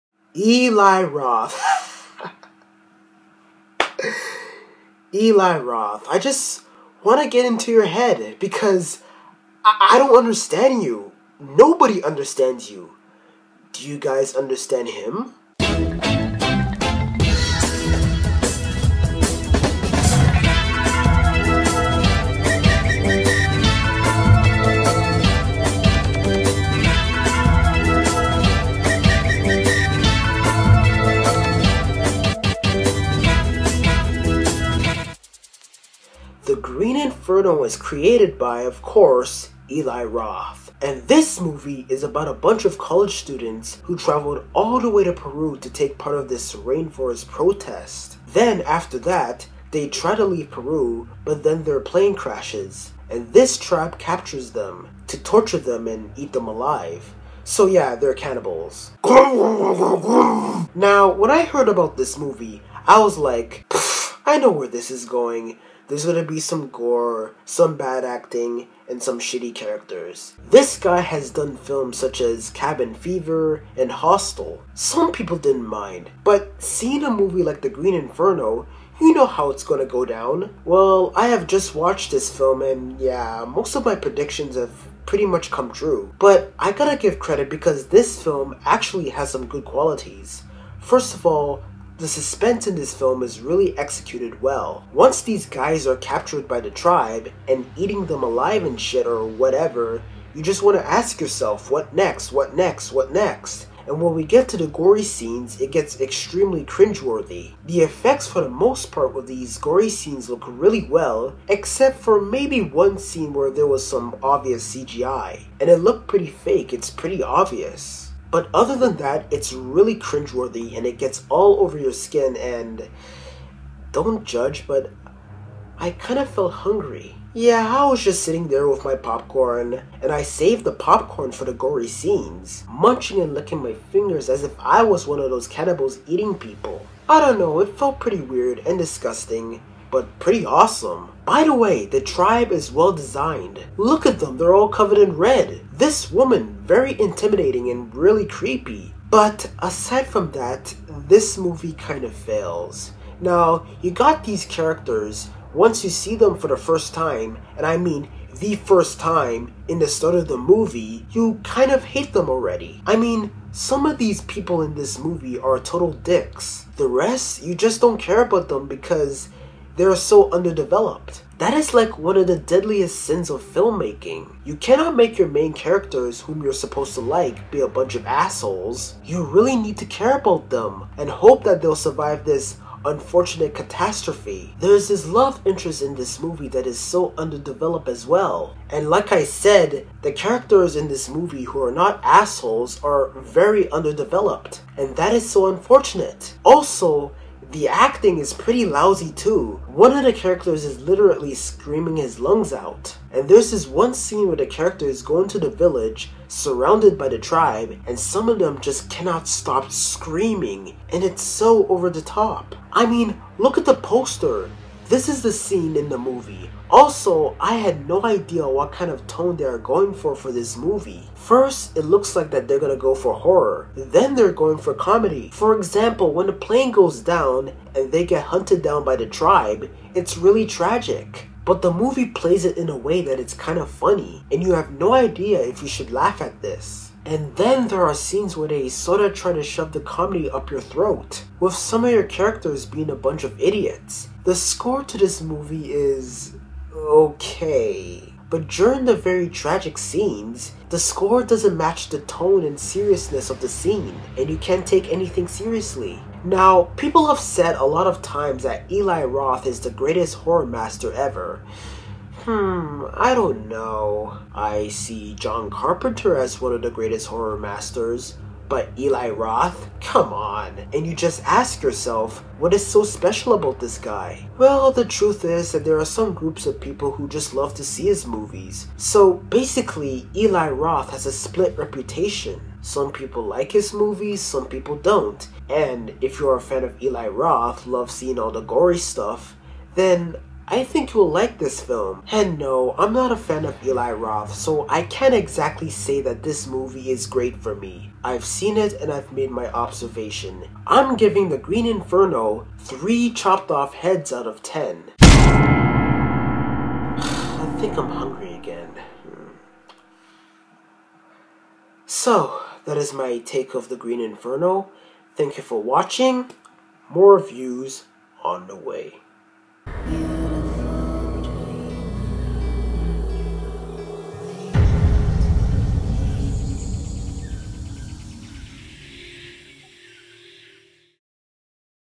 The Green Inferno Film Review - The Critic on Camera
New York college student Justine (Lorenza Izzo), a lawyer's daughter, meets a student activist named Alejandro (Ariel Levy) when he goes on a hunger strike on behalf of underpaid janitors. Smitten, Justine agrees to help Alejandro undertake his next project: to save the Amazon. She soon learns to regret her decision when their plane crashes in the Peruvian jungle and she and the rest of their group are taken captive by a tribe of hungry cannibals.